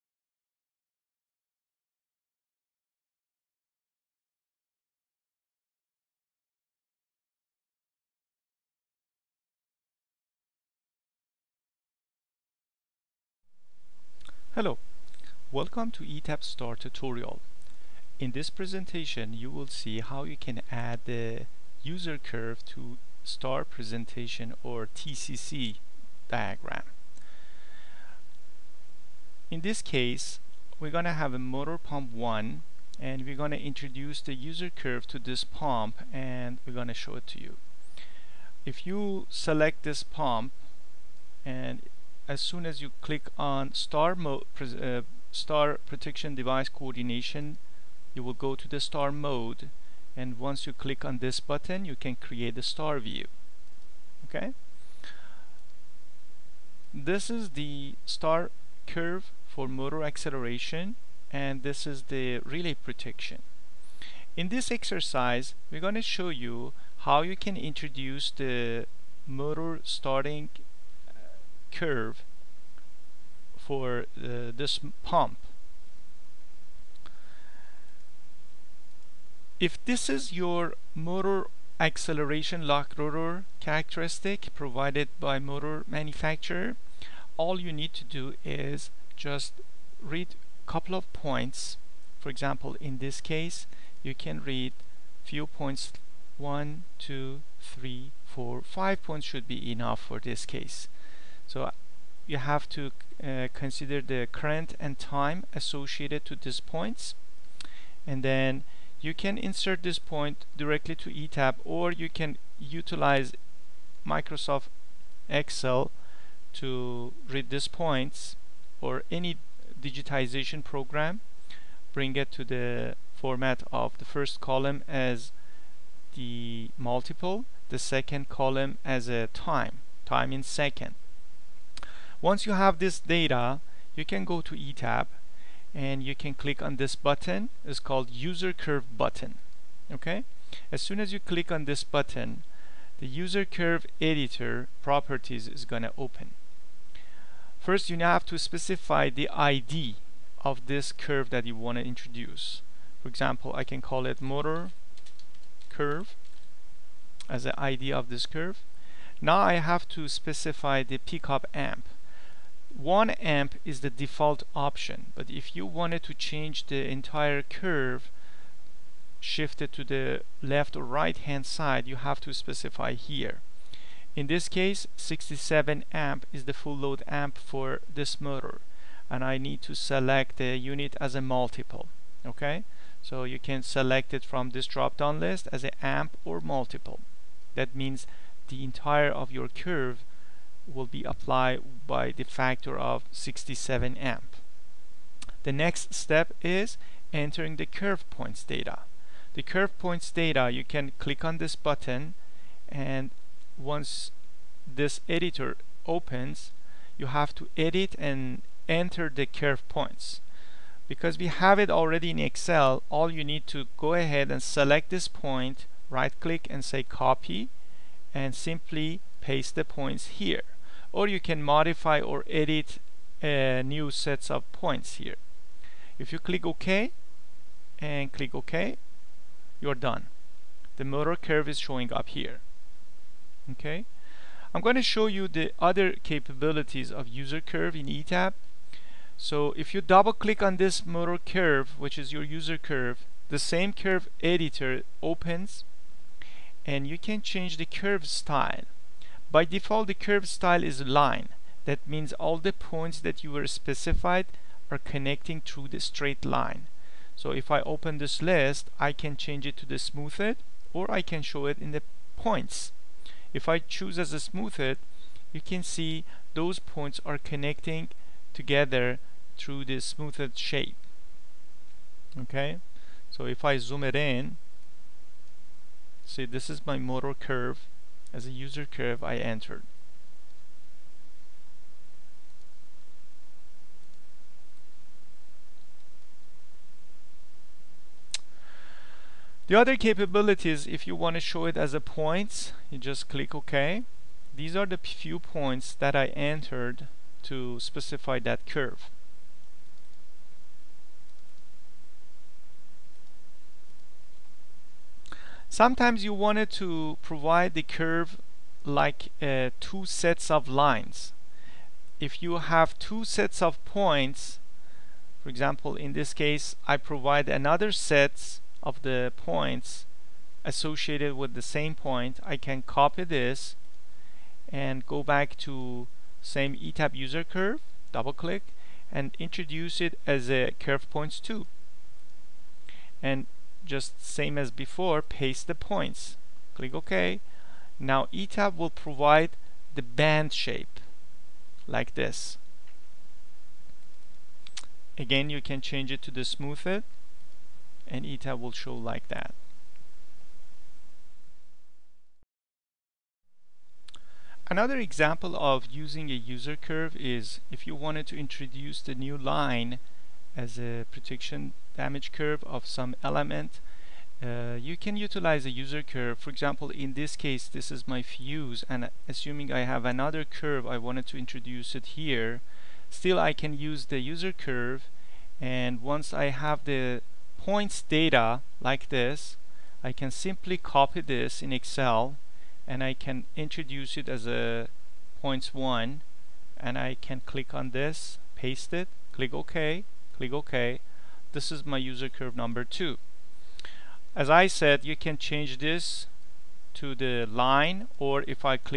ETAP Star - Making a User Curve - Part 1
There are cases when a user needs to draw specific curves onto a Star TCC presentation which is not included in the ETAP Star standard library. Applying ETAP "User Curve" is the solution for that.